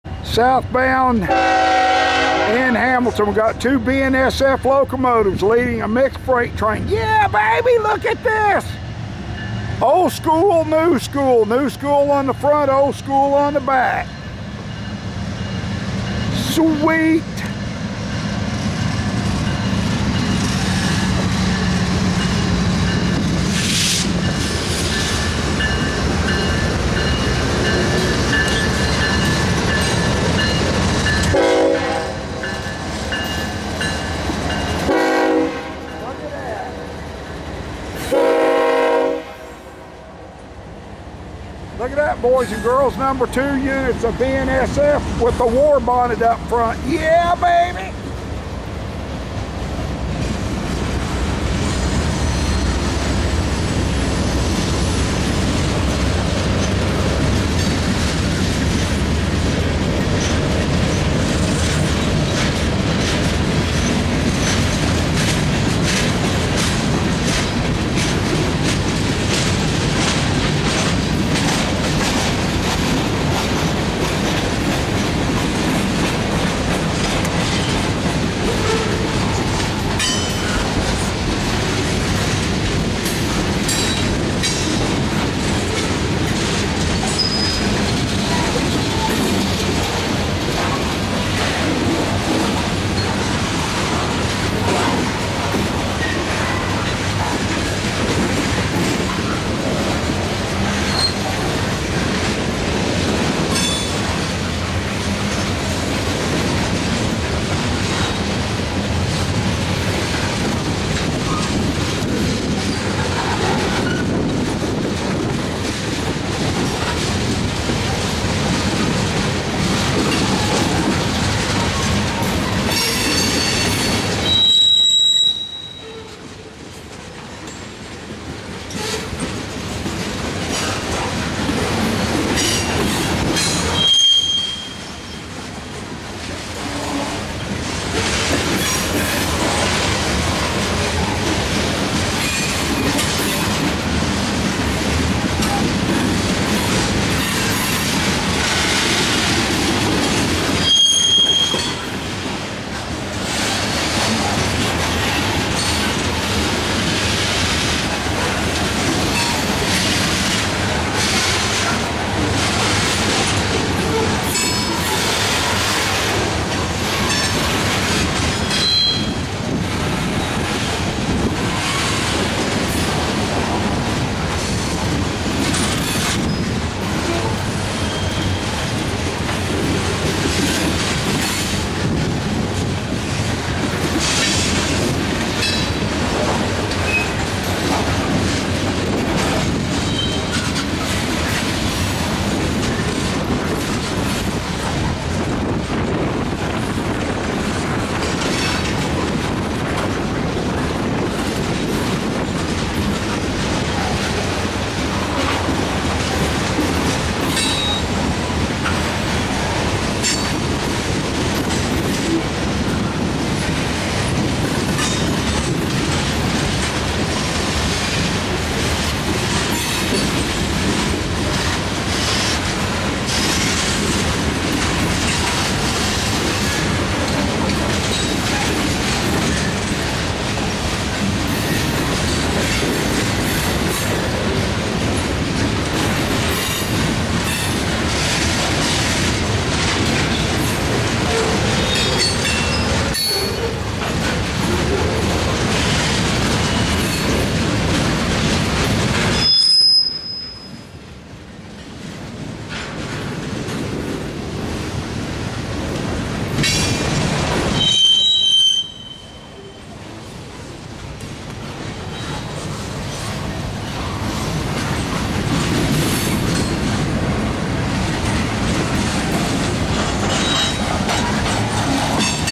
BNSF Running on Norfolk Southern Tracks Pulling Mixed Freight Train
BNSF Running on Norfolk Southern Tracks Pulling Mixed Freight Train in Hamilton, Ohio.  The first locomotive is painted in the new BNSF color scheme.  The trailing locomotive has the older style war bonnet paint scheme.